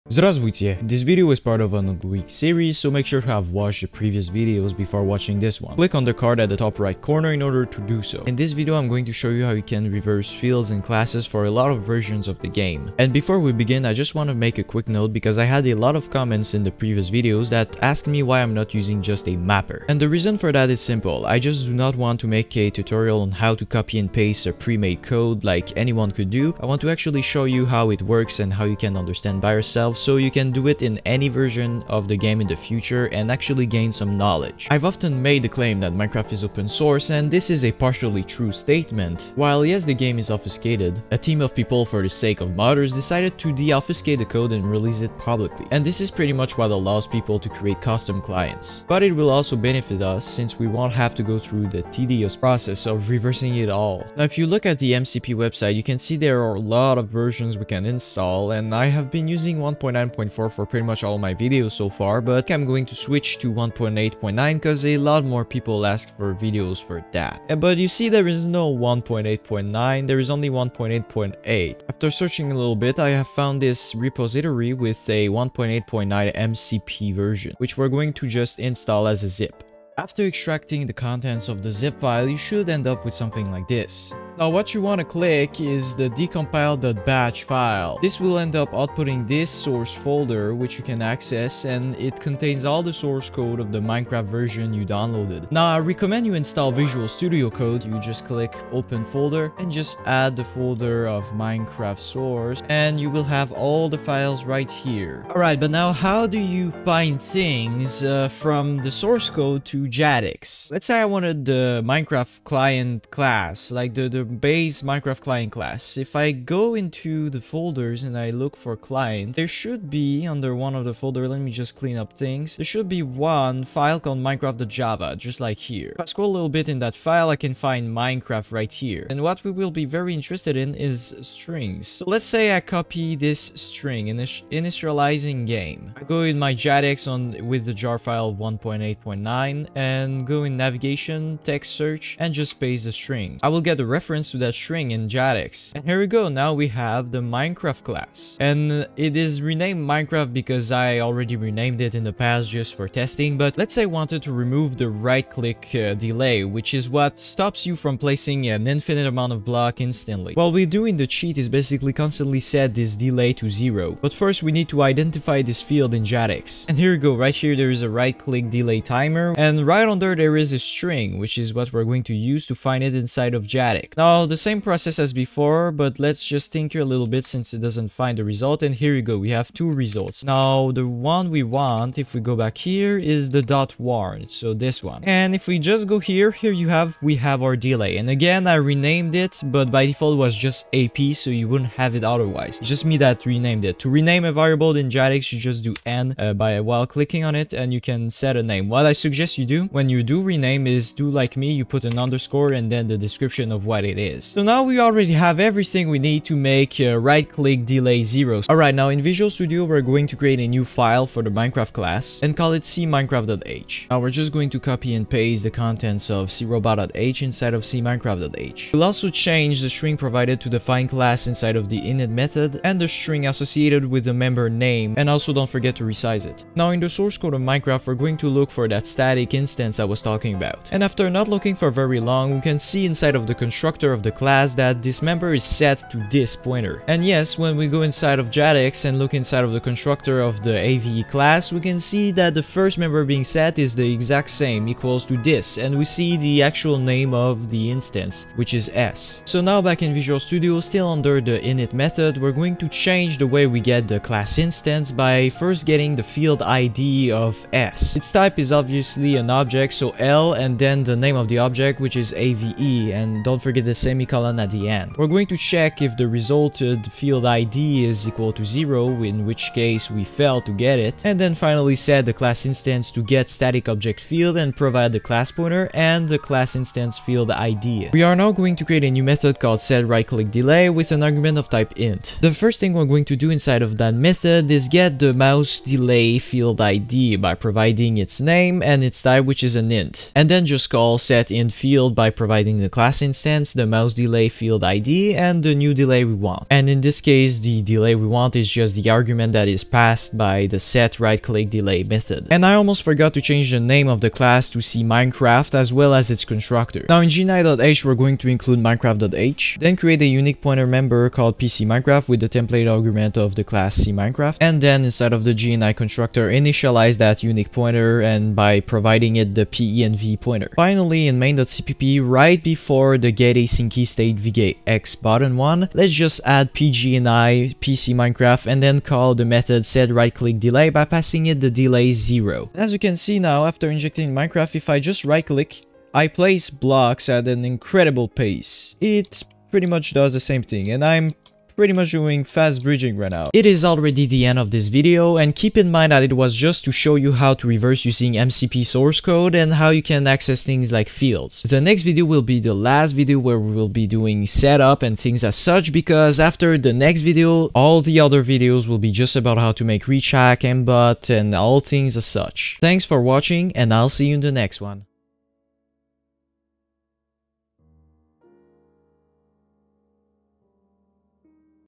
REVERSING - CODE A MINECRAFT JNI HACKED CLIENT
In this video we will cleanup the code and make a basic auto clicker.

⏰Chapters⏰
00:00 Intro
00:54 Download MCP
01:32 Setup vscode
01:45 Coding
05:31 Injection
05:43 Outro

Shout out to @kapitcheGHT for the thumbnail ⭐